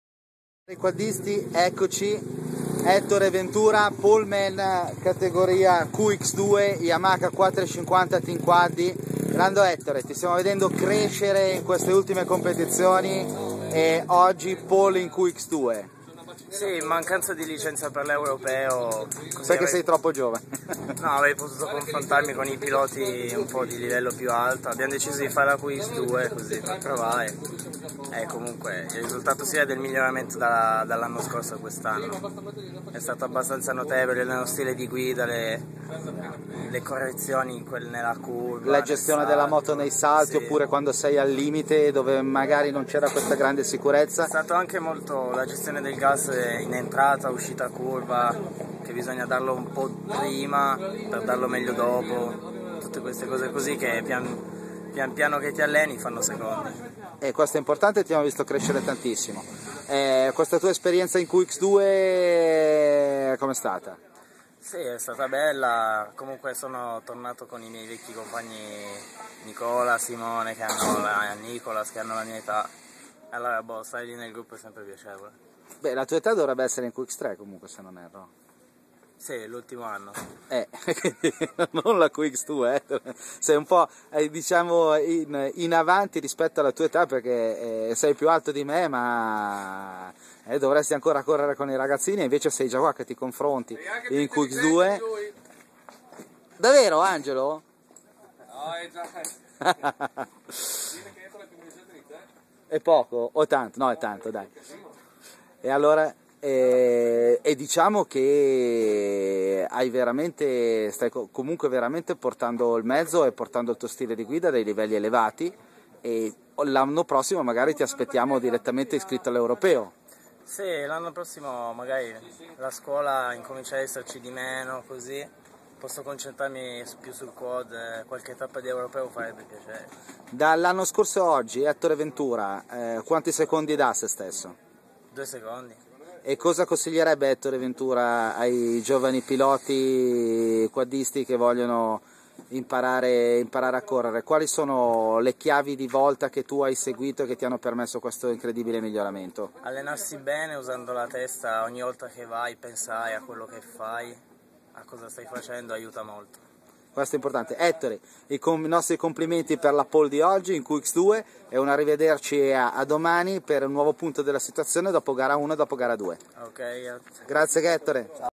2012-06-09_quadcross_sanseverino_crono_qx2_ventura.3gp
Quad Cross 4° prova campionato italiano e campionato europeo Quad Cross prova italiana a San Severino Marche - Videointerviste de ilFero direttamente dai box ai grandi campioni e videocronache dirette delle gare - diritti di Qiaddisti.it - in questo videoarticolo Ettore Ventura, intervista dopo le cronometrate di sabato - Video articolo completo a - Radio Box di Quaddisti.it